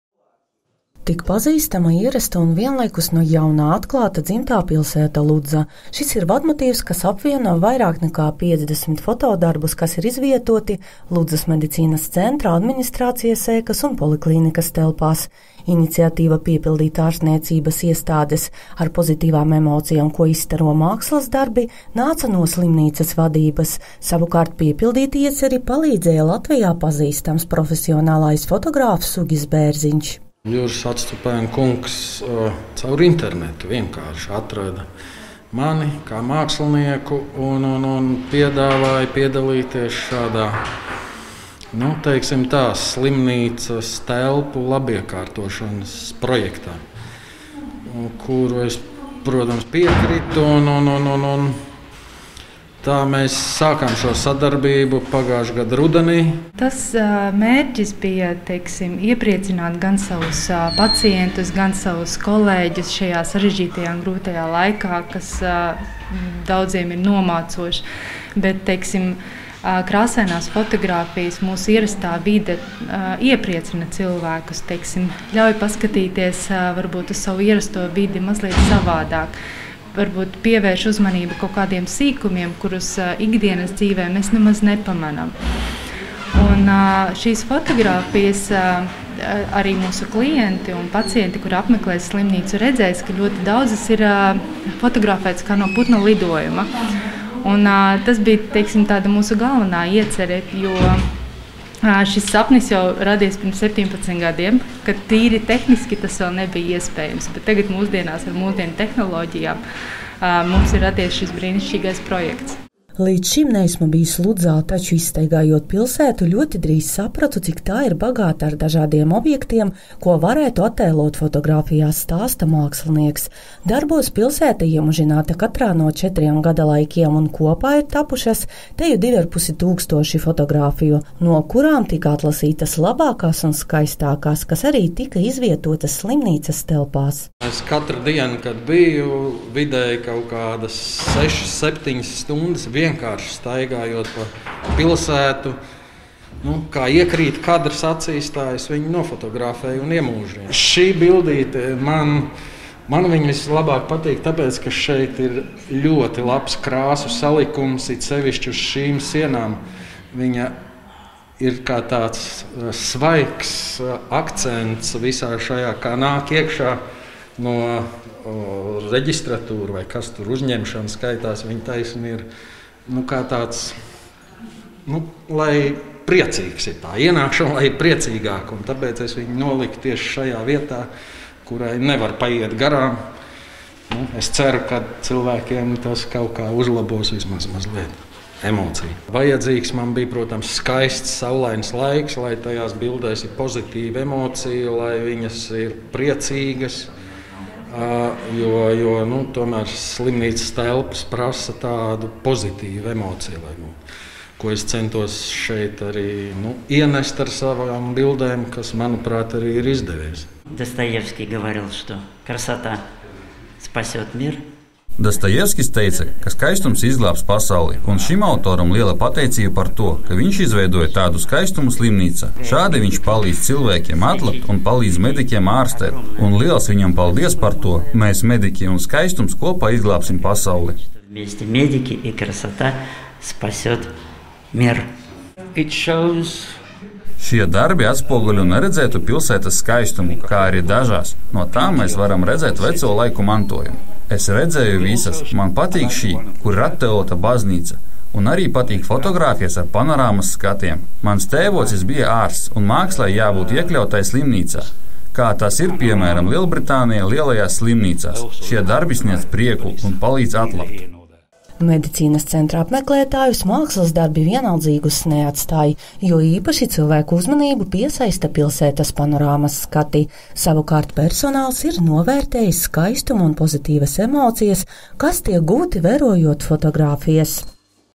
Ludzas medicīnas centrā ienāk māksla
Rūpējoties par personāla un apmeklētāju labsajūtu, Ludzas medicīnas centra telpās tika iekārtota foto galerija, ko veido vairāk nekā 50 fotogrāfijas ar Ludzas skatiem. Ar saviem darbiem autors, Latvijā pazīstams fotomākslinieks Uģis Bērziņš aicina skatītājus pavērot pilsētu no neierastiem skatu punktiem un no jauna atklāt tās skaistumu